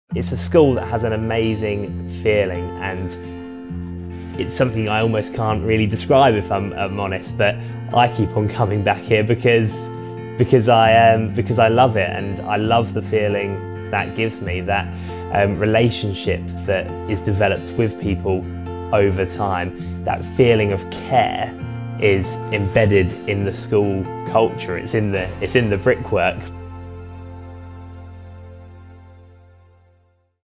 Mr Turner loved Fearnhill so much, he came back to teach!
A former student here and now Head of KS3, Mr Turner explains just what Fearnhill means to him.